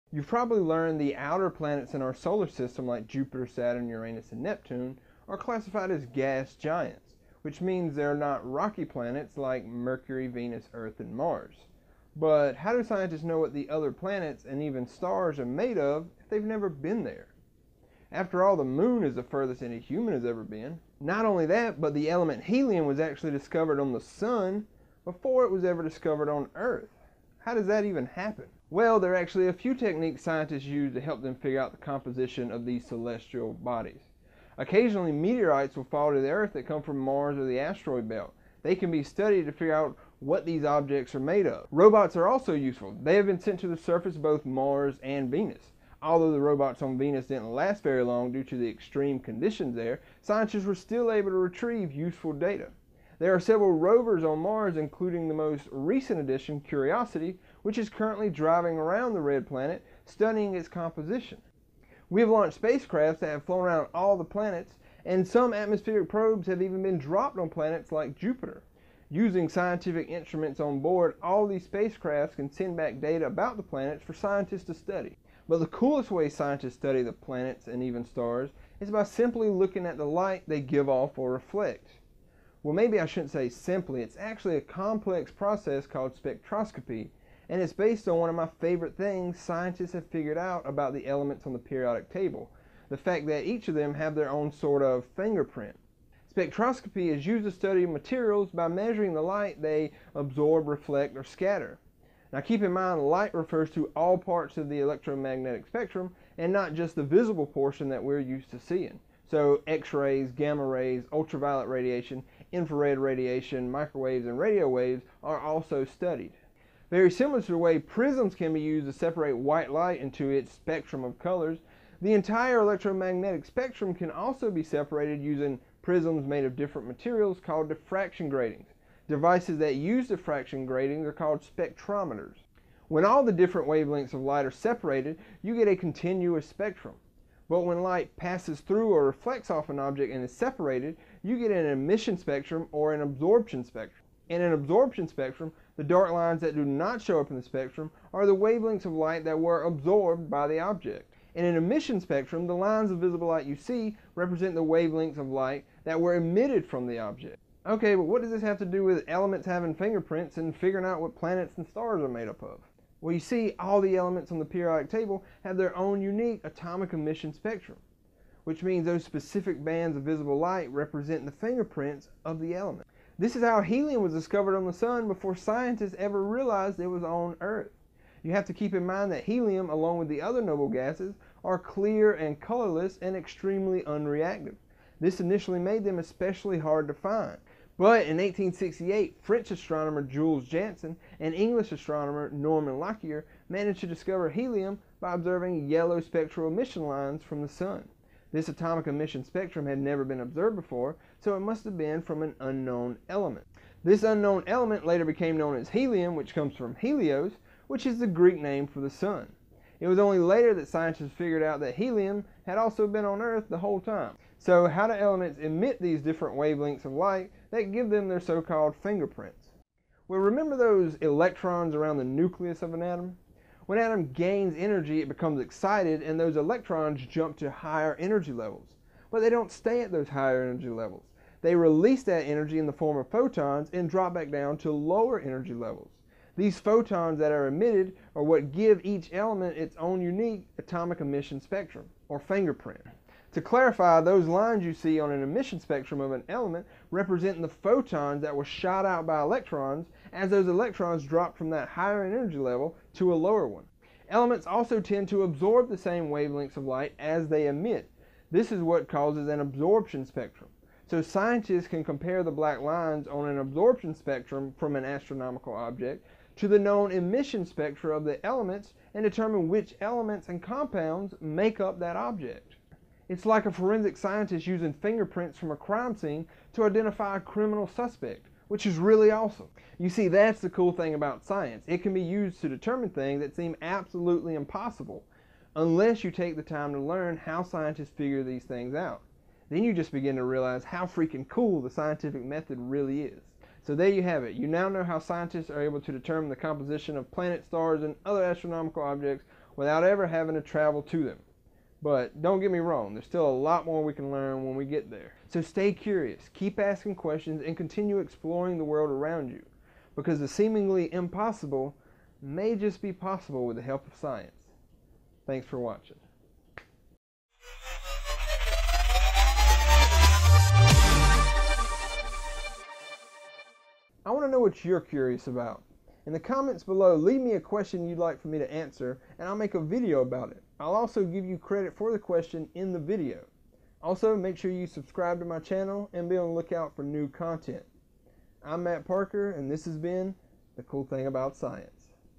How Do Scientists Know What Planets And Stars Are Made Of?
Ever wonder how scientists know what the other planets and stars are made of without ever having been to them? Well did you know that each of the elements have their own unique "fingerprint"? Learn how these "fingerprints" are studied and used to determine the composition of astronomical objects.